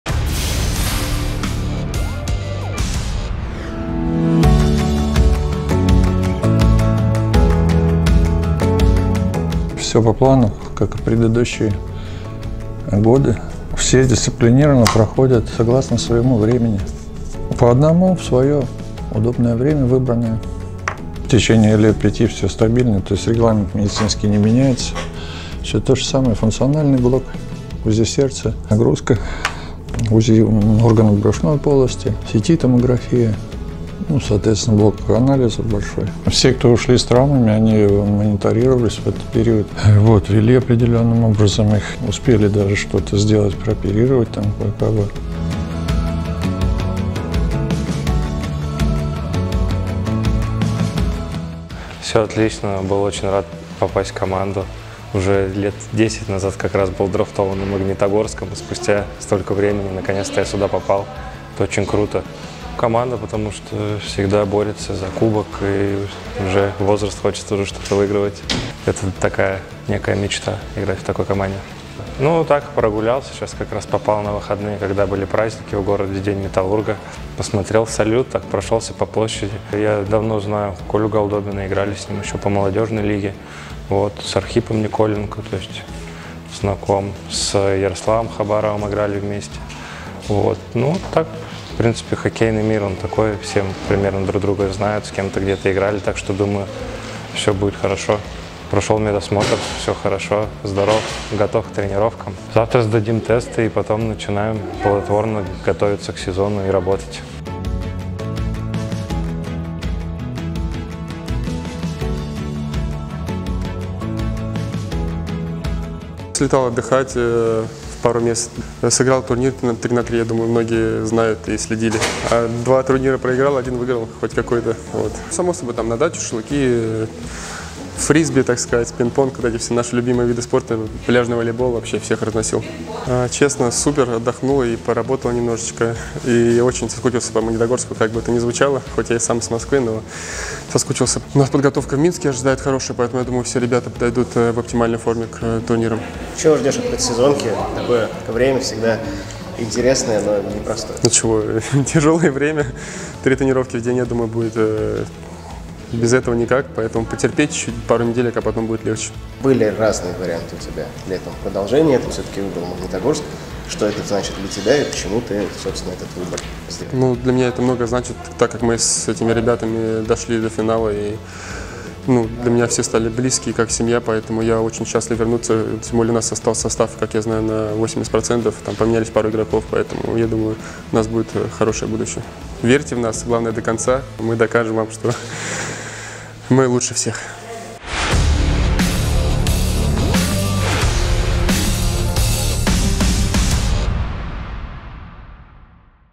«Металлург» вышел из отпуска и сдаёт медицинские тесты
С чего начинается предсезонка? Конечно, с прохождения медицинского осмотра – всевозможных исследований и тестов. По словам медицинского штаба «Металлурга», все процедуры проходят в привычном режиме. Мы пообщались с доктором команды и побывали в медицинском центре вместе с игроками. Смотрите видео!